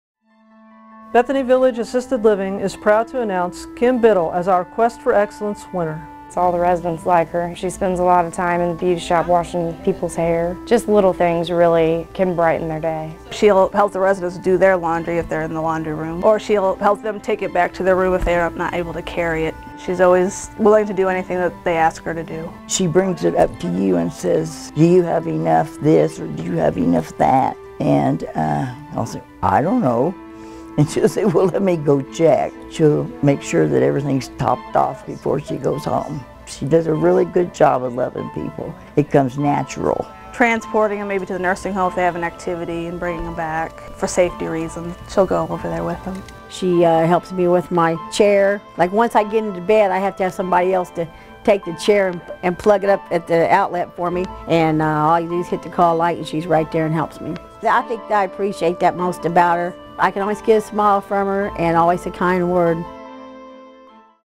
Bethany Village Assisted Living, Quest for Excellence Winner 2014, ASC
American Senior Communities announces the 2014 Quest for Excellence winner (Employee of the Year) from Bethany Village Assisted Living.